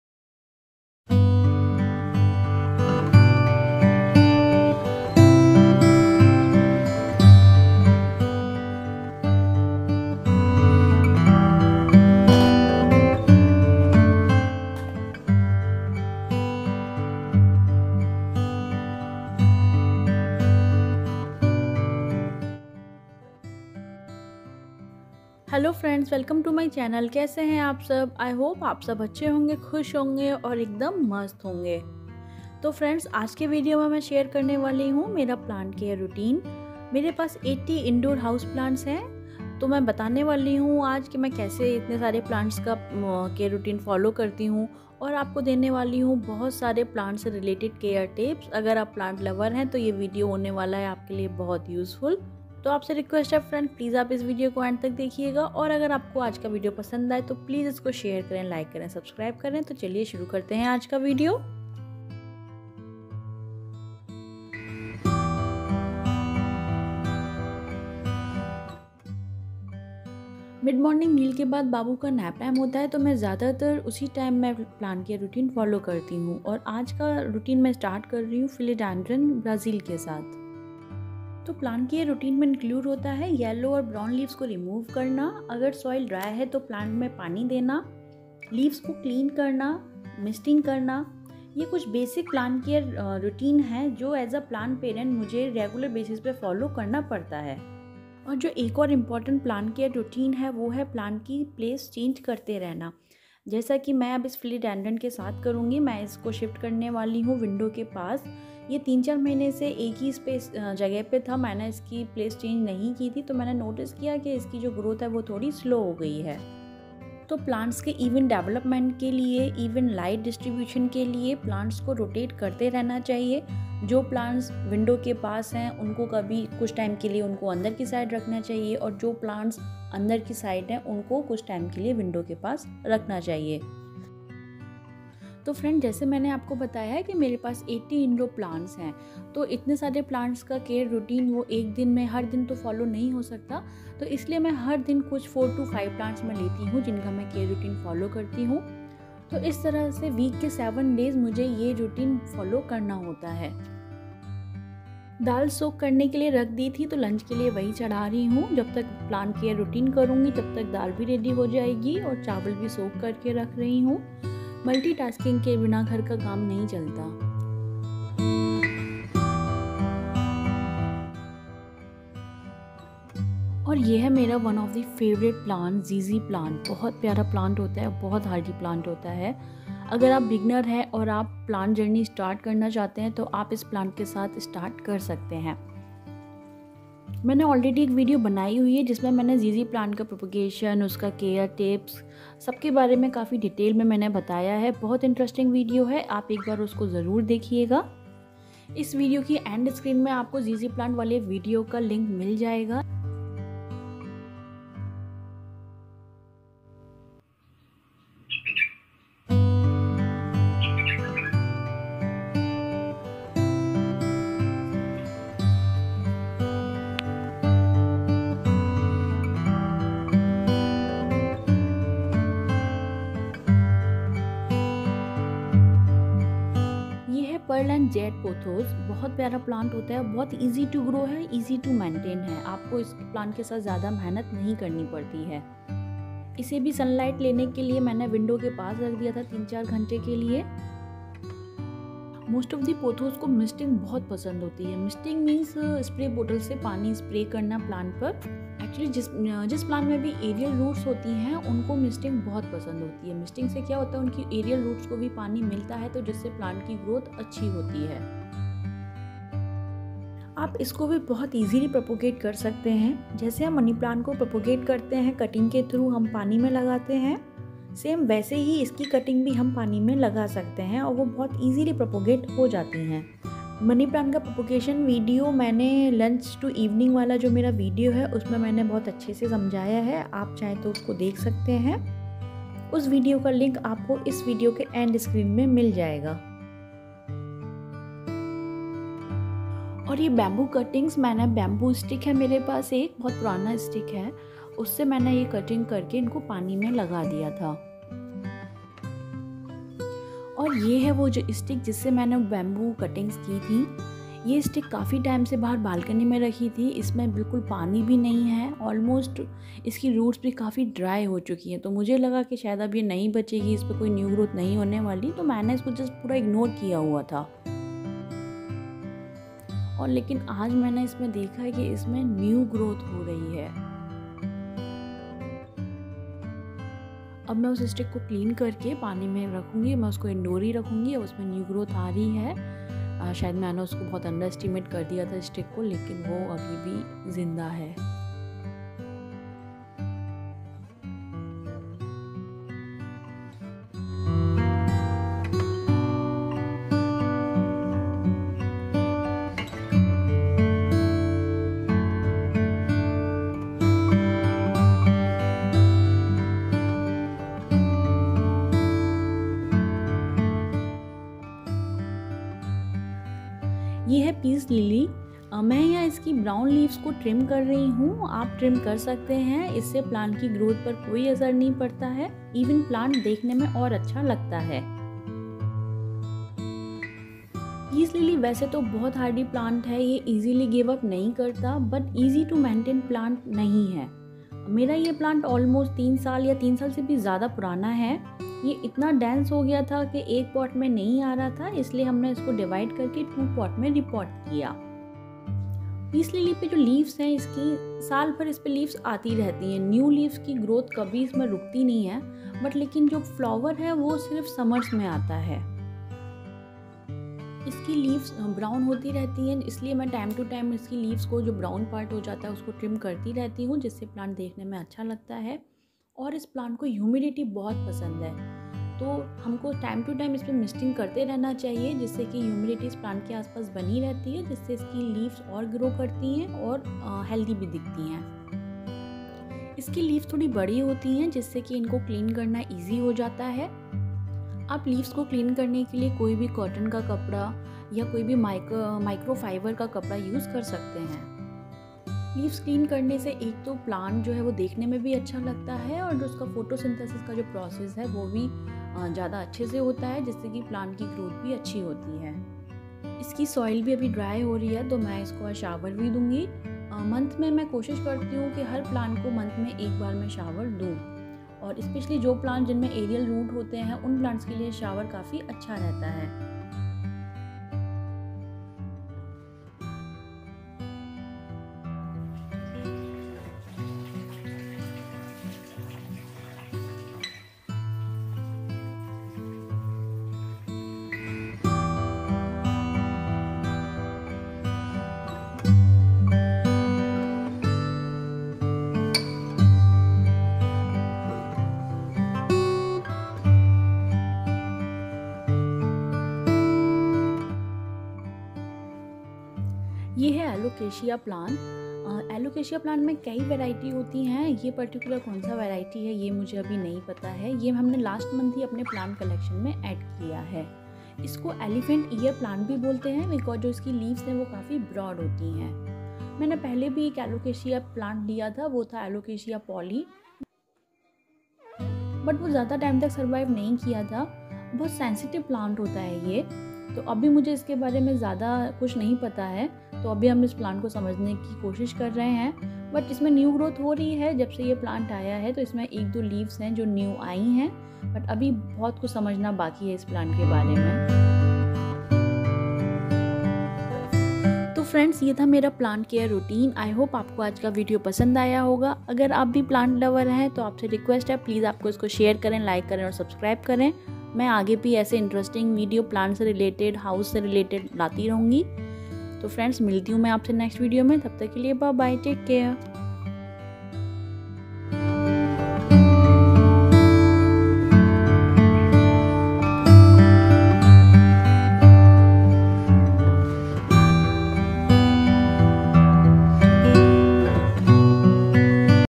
Living Room Garden| Gardening Ideas For Home | Indoor Plants Care Tips
Hello Friends
In this video I am going to share my plant care routine of 80 indoor plants.

Thanks